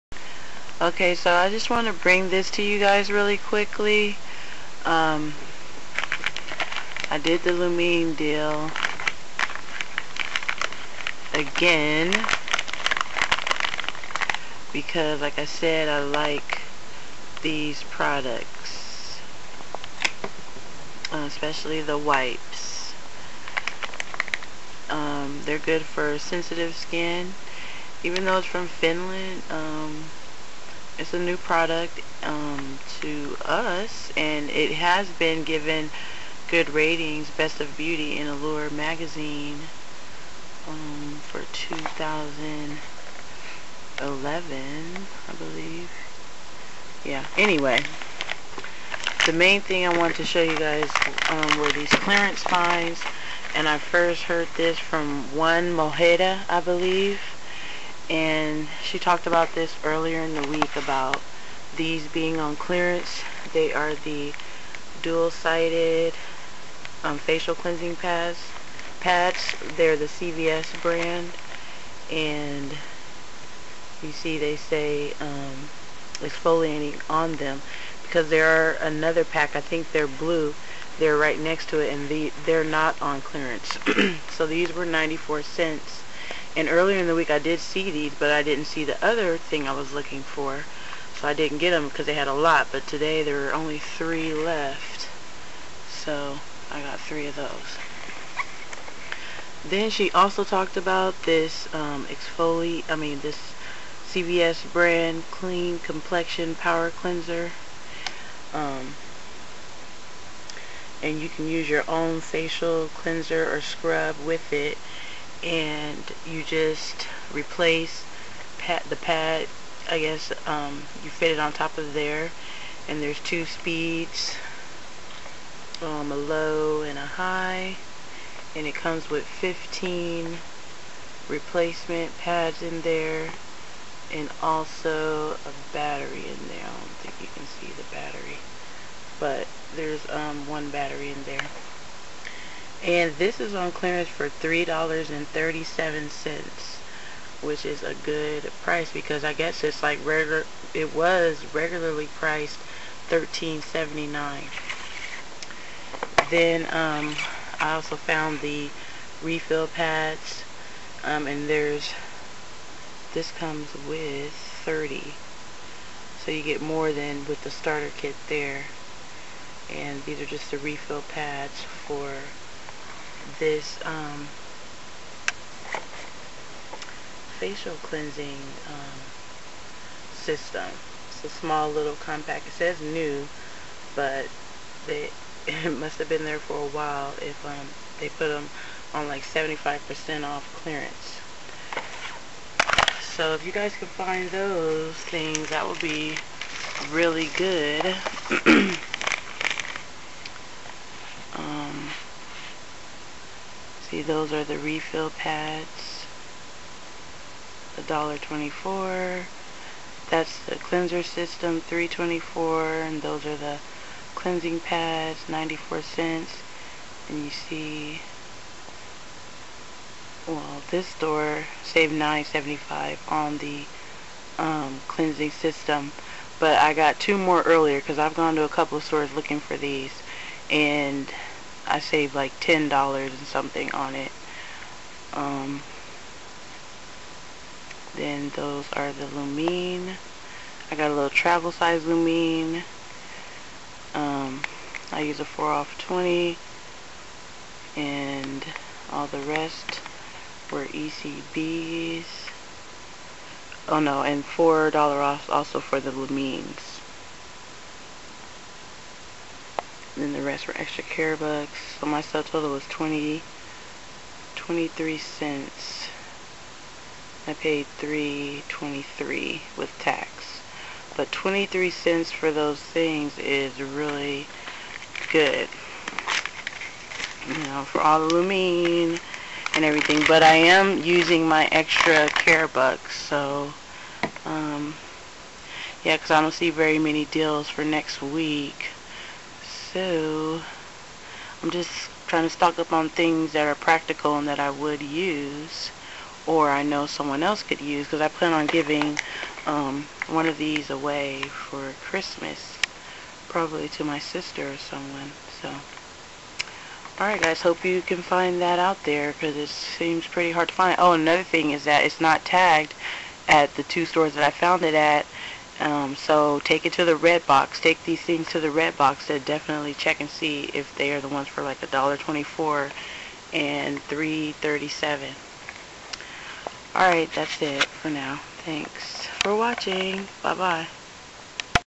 CVS Clearance Finds and Lumene 12-1-2012
I am still doing shopping at CVS this week and I know I need to stop but I am a sucker for deals!  I hope you all can find these items because they are a good idea for stocking stuffers.  I did get back another $10 ECB for the Lumene.  Let me know if you have any questions.
Thanks